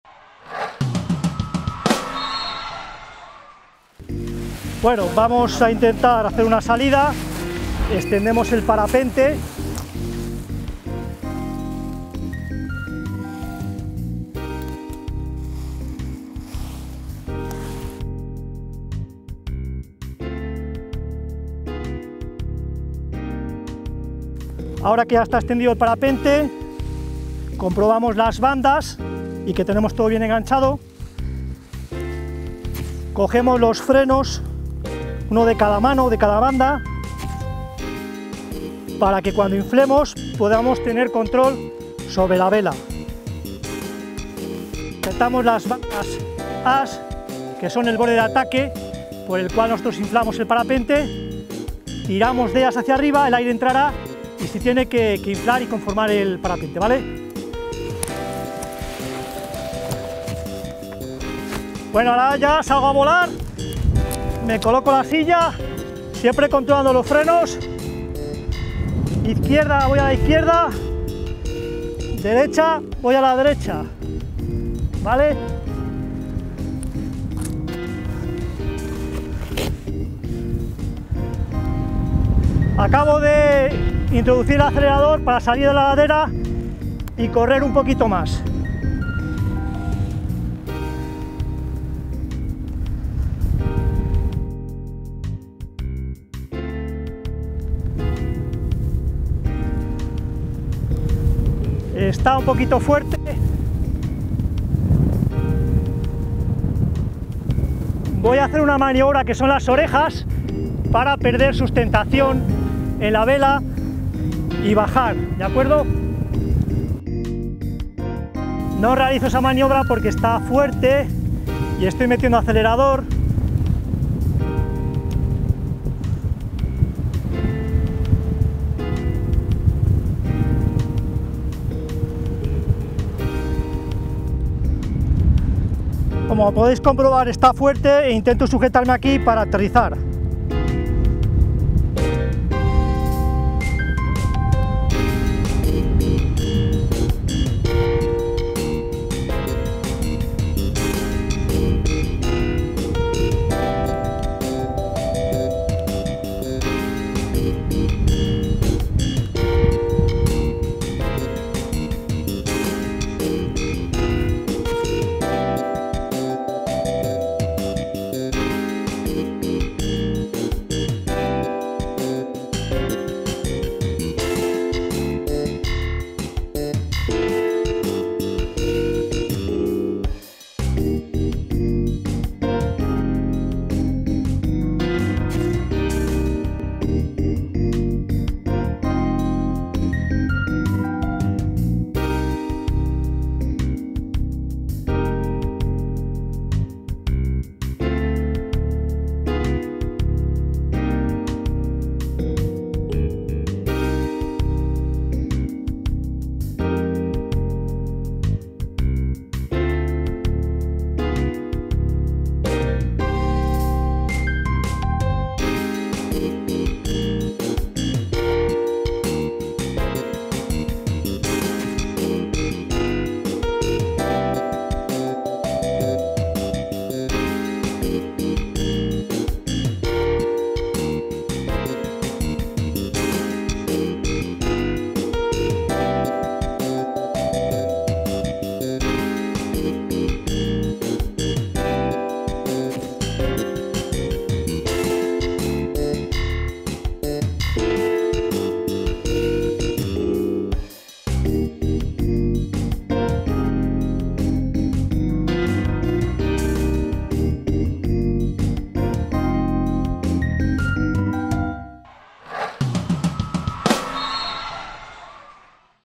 PARAPENTE 6. Salir a volar
El piloto de parapente Mario Cadenas nos enseña como volar en parapente. Vemos los pasos a seguir para comenzar el vuelo, la forma de manejar el paracaidas en el aire y nos muestra también el aterrizaje.